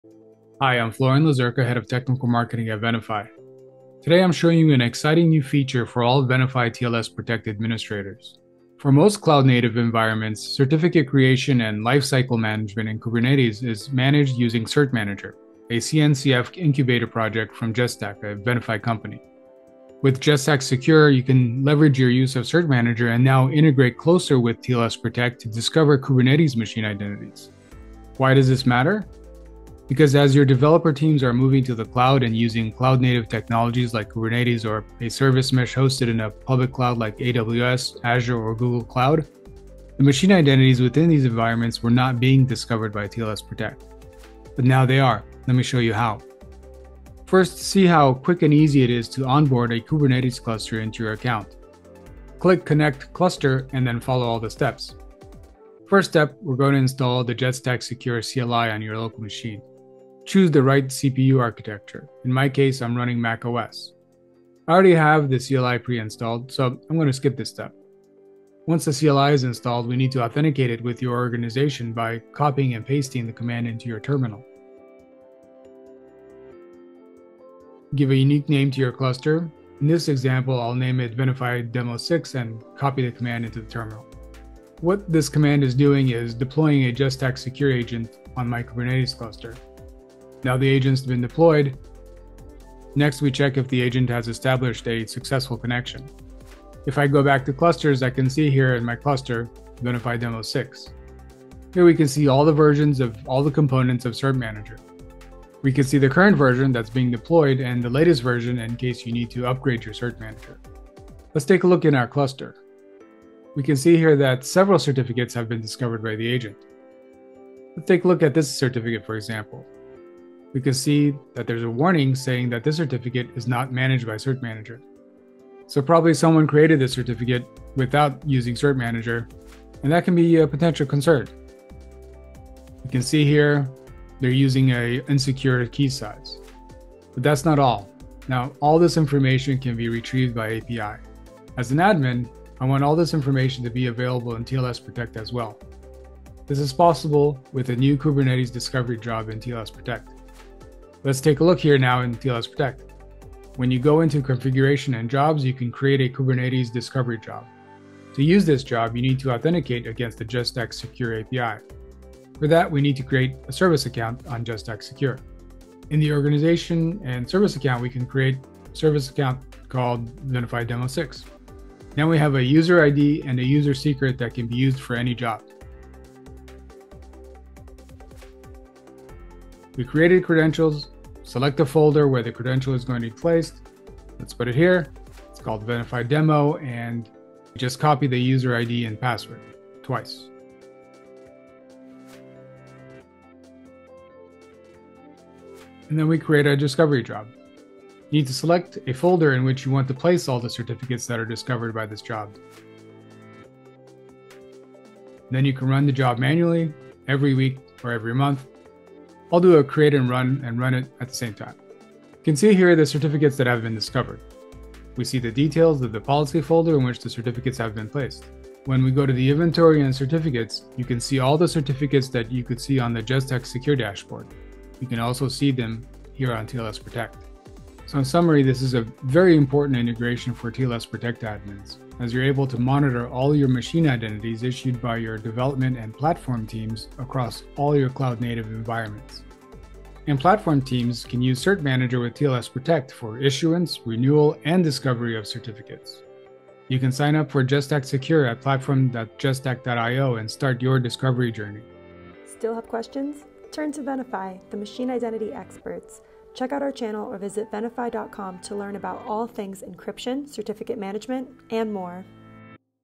Kubernetes Discovery from TLS Protect Data Center | Technical Demo, Venafi
All TLS Protect admins now have access to a fantastic new feature. As we know, certificate creation and lifecycle management in Kubernetes is done with cert manager. Now, Jetstack Secure allows you to leverage cert manager and integrate with TLS Protect to discover Kubernetes machine identities.

Why is this so exciting? As developers make the shift to primarily cloud native technology, machine identities within these environments are now discoverable and manageable by TLS Protect Cloud (where previously, they were not). Watch this short demo to learn how.